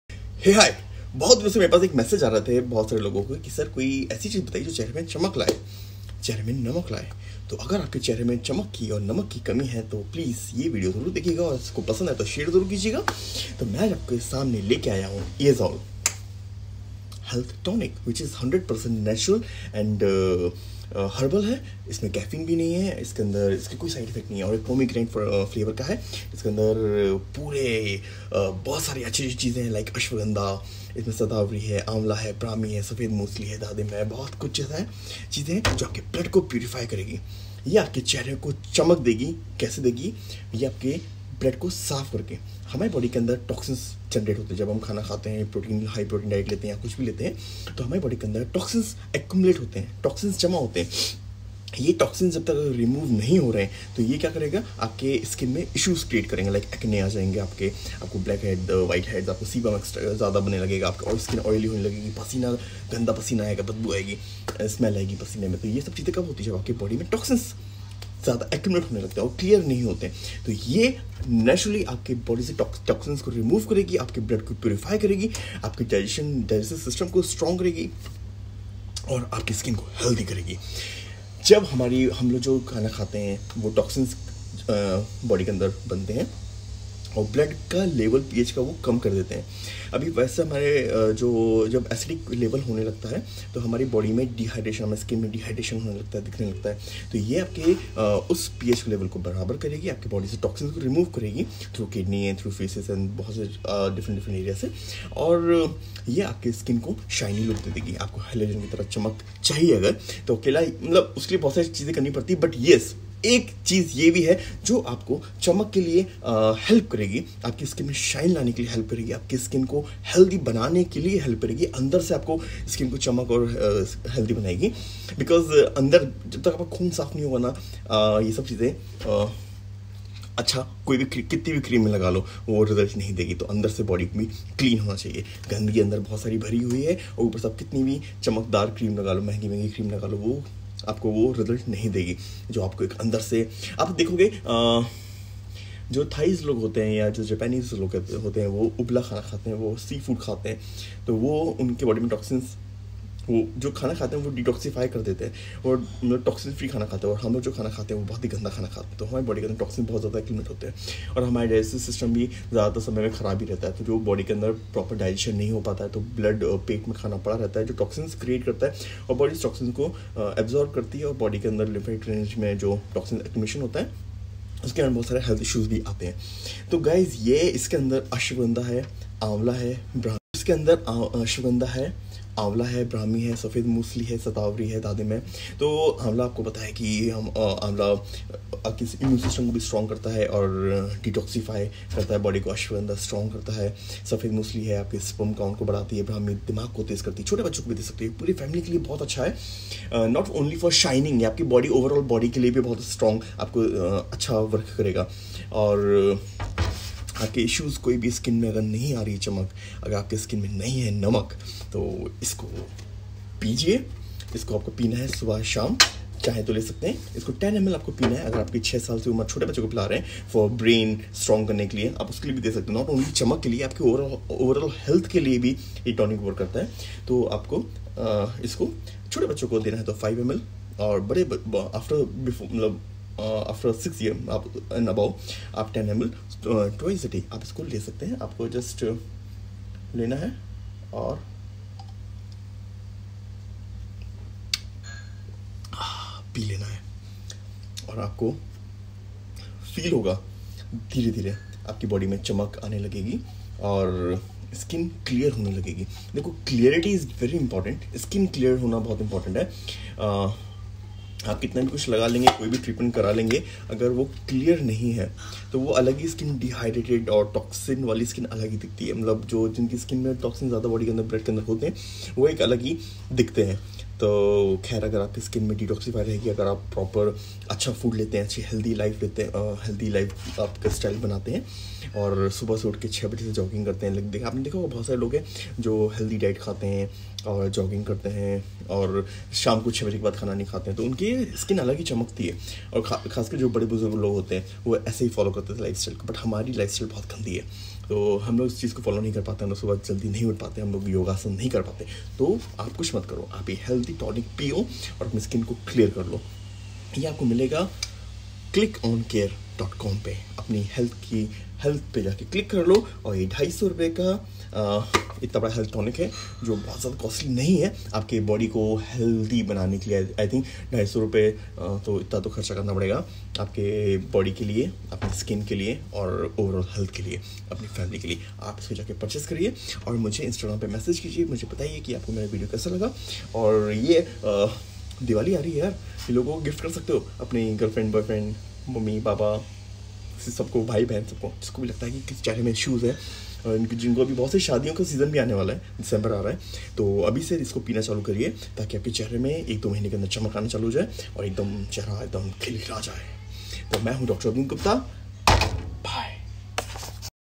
Get Glow and Shiny Skin Naturally from within | Eazol | Dr Abhinit Gupta
Eazol has Amla (Gooseberry) in it, Amla is one of the most ancient fruits now recognized as one of the richest natural sources of vitamin C.

One of the most remarkable and popular qualities of amla is its impact on skin. Amla is loaded with skin-friendly Vitamin C which can do wonders for all your skin woes

Best product for Boosting Immunity and Appetite

Safed Musli is in building and maintenance of our muscles which becomes essential

Eazol Health Tonic provides Adequate Nutrition, Helps In Maintaining Overall Health of all and is good for Diabetic patients also

pomegranate is a rich source of vitamins and minerals. It is rich in antioxidants

Eazol में आंवला (Gooseberry) होता है, आंवला सबसे प्राचीन फलों में से एक है जिसे अब विटामिन सी के सबसे अमीर प्राकृतिक स्रोतों में से एक माना जाता है।

आंवला का सबसे उल्लेखनीय और लोकप्रिय गुण त्वचा पर इसका प्रभाव है। आंवला त्वचा के अनुकूल विटामिन सी से भराजो आपकी त्वचा की सभी समस्याओं के लिए चमत्कार कर सकता है

बेस्ट प्रोडक्ट फॉर बूस्टिंग इम्युनिटी एंड एपेटाइट होता है

eazol Health Tonic पर्याप्त पोषण प्रदान करता है, सभी के समग्र स्वास्थ्य को बनाए रखने में मदद करता है

अनार विटामिन और खनिजों का एक समृद्ध स्रोत है। यह एंटीऑक्सिडेंट में समृद्ध हैऔर मधुमेह के रोगियों के लिए भी अच्छा है।